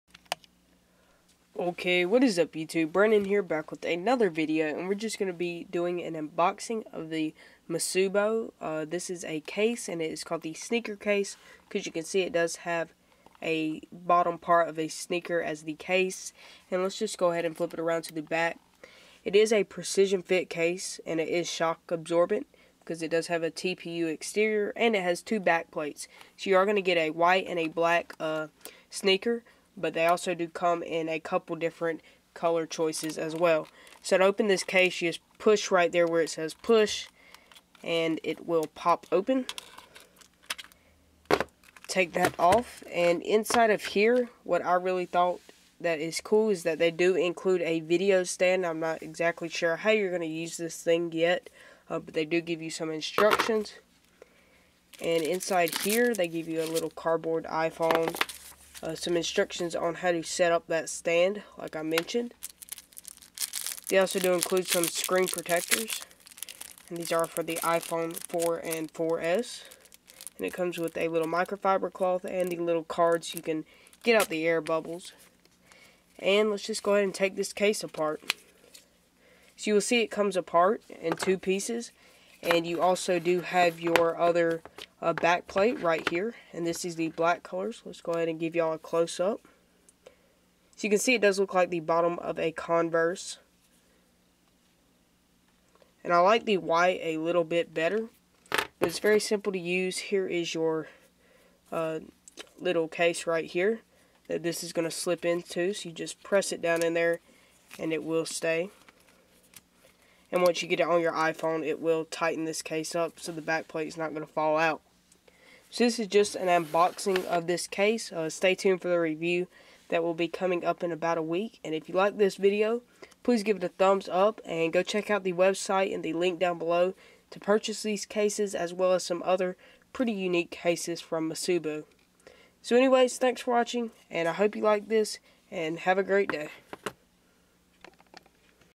Musubo Sneaker Case for iPhone 4/4S Unboxing
Slip your iPhone into ultimate protection with Musubo's personalizable Sneaker Case. A shock absorbing, grip-enhancing exterior is complimented by two interchangeable "sole" back-plates to mix & match your style. Precision fit for the iPhone 4/4S with folding video stand added for entertainment on the go.